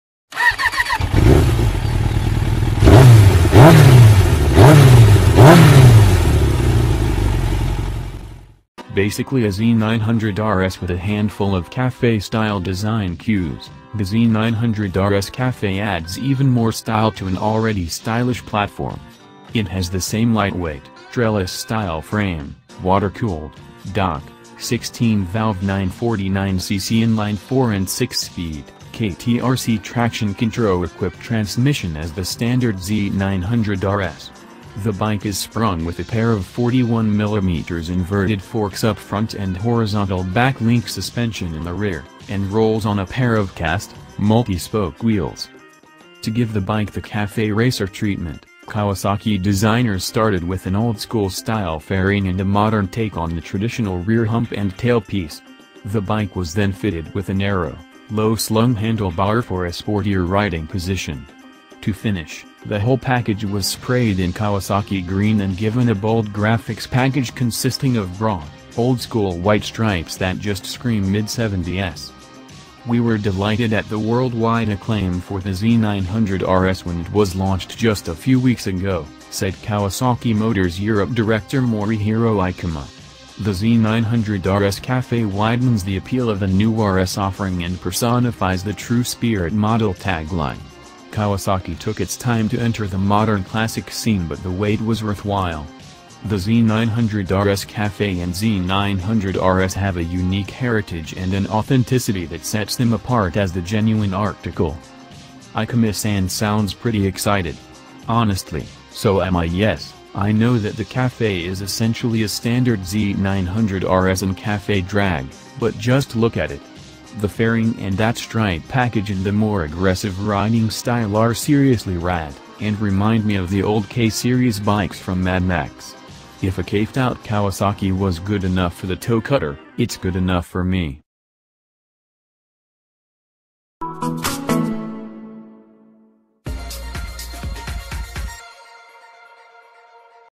The New ! 2017 kawasaki z900rs retro
A retro take on a retro take

Remember how we were all excited when Kawasaki dropped the Z900RS at the Tokyo Motor Show? Well, apparently someone at Kawasaki heard we like retro-styled bikes, so they retro-ed up the throwback Z900RS even more with a bikini fairing and some other bits and bobs to produce the sexy Z900RS Cafe.

kawasaki z900rs retro, z900rs ,kawasaki z900rs review,kawasaki z900rs 2017 review,new kawasaki ,kawasaki z900rs 2018,new kawasaki z900rs ,kawasaki